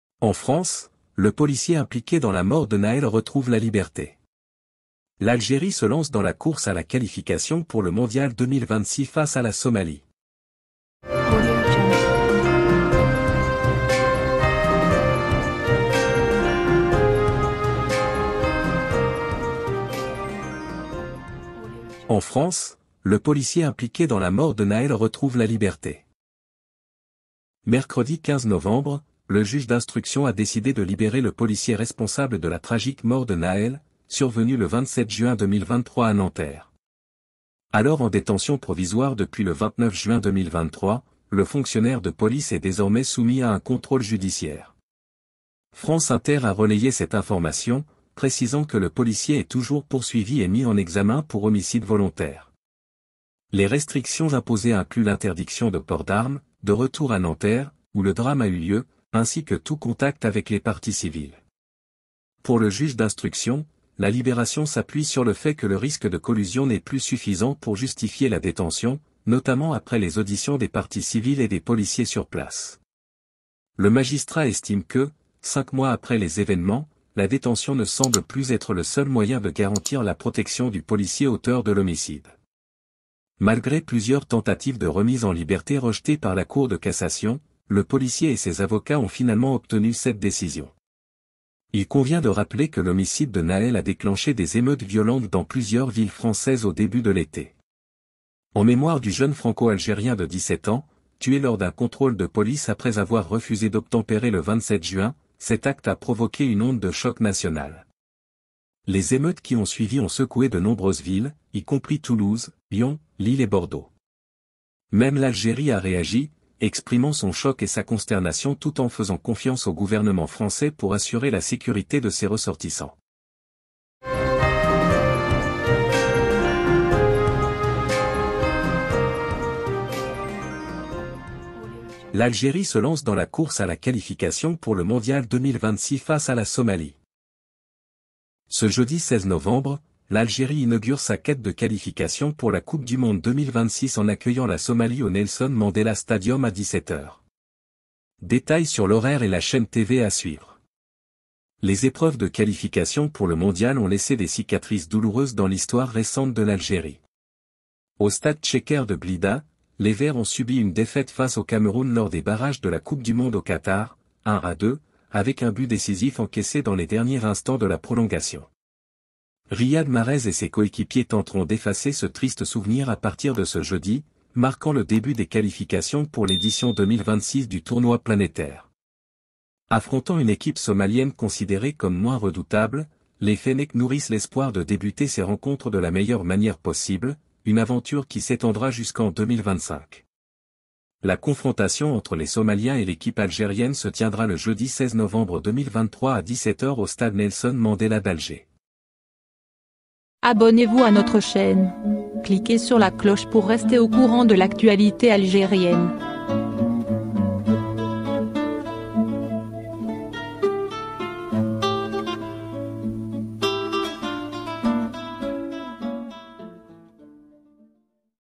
😱Policier Responsable de la M'ort de Nahel Libre| Mondial2026: Algérie-Somalie: Horaires & Chaînes🕒📺
Découvrez les derniers rebondissements dans l'affaire tragique de Nahel, où le policier impliqué retrouve la liberté. Analyse des décisions judiciaires et réactions publiques face à cette libération controversée.
Découvrez en détail le coup d'envoi de l'Algérie dans sa campagne de qualification pour la Coupe du Monde 2026 contre la Somalie. Analyses, attentes, et les premières impressions de cette rencontre cruciale. Restez avec nous pour suivre le parcours des Fennecs vers le Mondial !